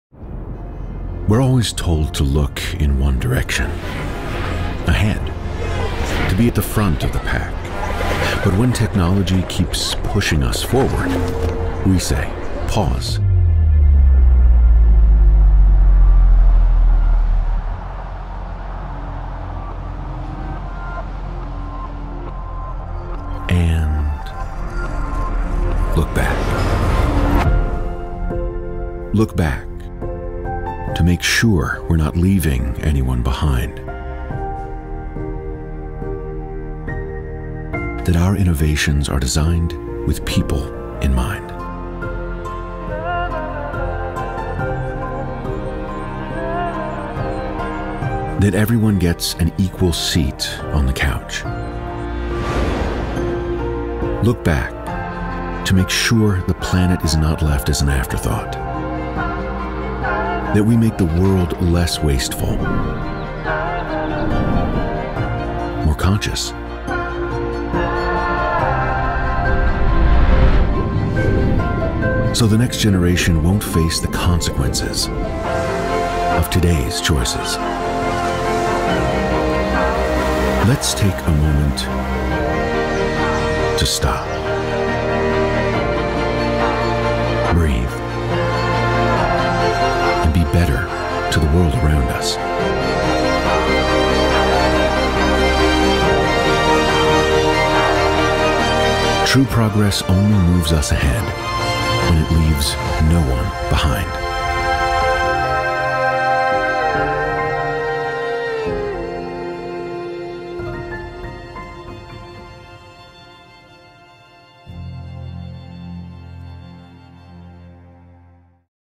Screens for All: Leave no one behind | Samsung
In 2020, we introduced a vision of Screens Everywhere.
In 2021, we are expanding our vision to Screens for All.

Screens for All encapsulates our mission to innovate to serve the needs of all, from our environment to the people living in it. In 2021 and beyond, we want to show how true progress leaves no-one behind. Screens Everywhere. Screens for All.